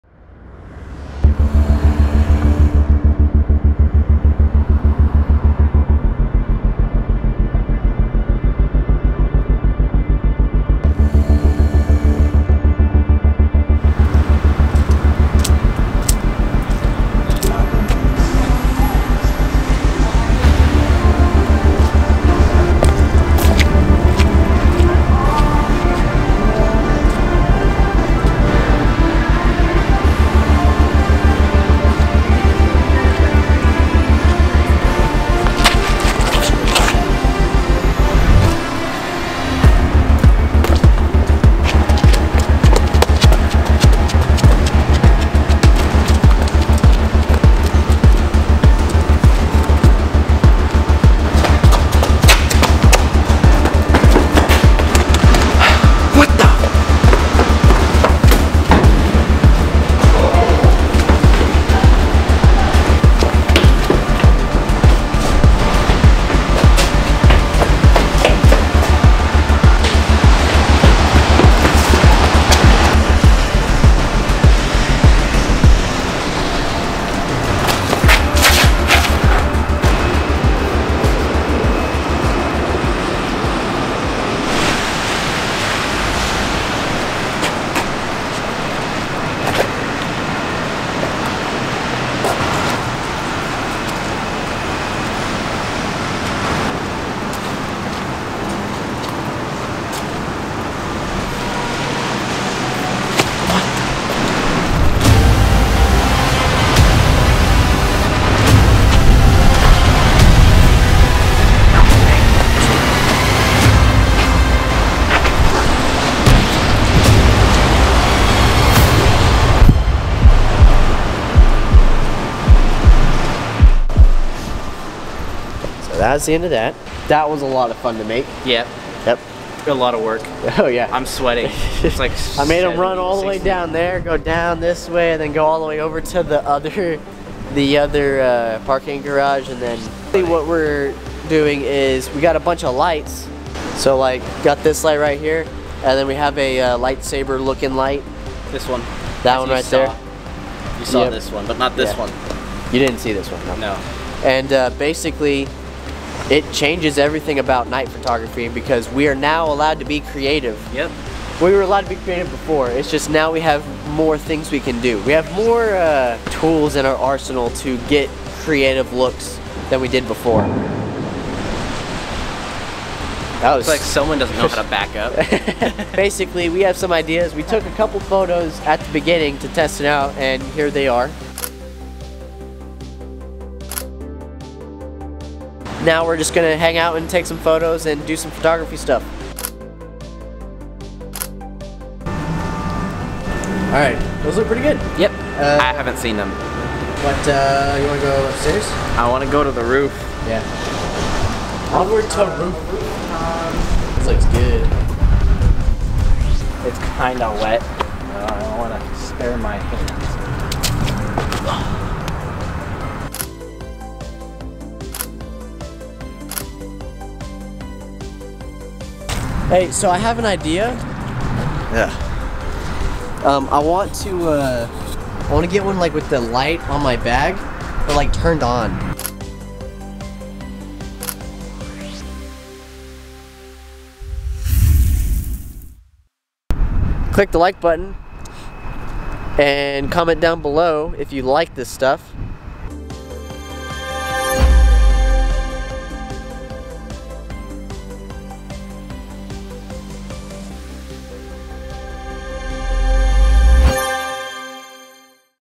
The best way to take photos at night
This changes everything about night photography. follow me as I explore a parking garage at night with some lights and see what we can create.